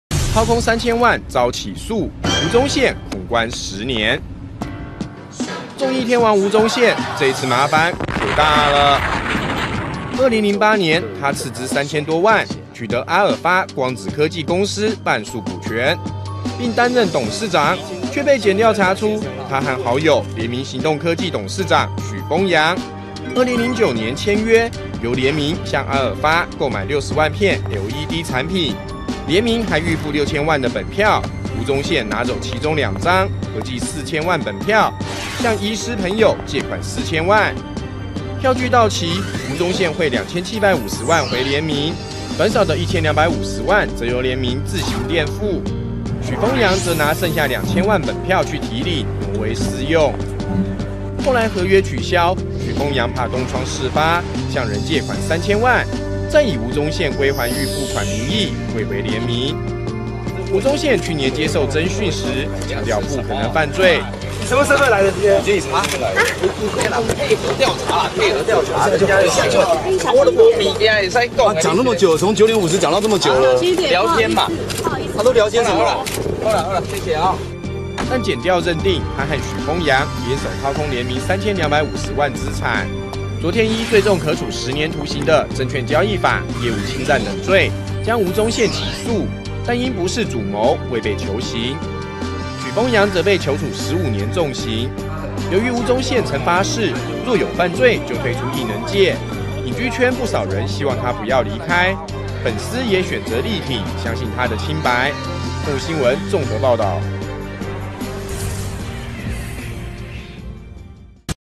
掏空3千萬遭起訴 吳宗憲恐囚10年
【綜合報導】綜藝天王吳宗憲投資LED節能產業阿爾發光子科技公司，不但虧損連連，還被檢調查出涉嫌和上櫃公司聯明行動科技董座許豐揚勾結，利用假交易共同掏空聯明3250萬元資產，昨被依最重可處10年徒刑的《證券交易法》業務侵占等罪起訴。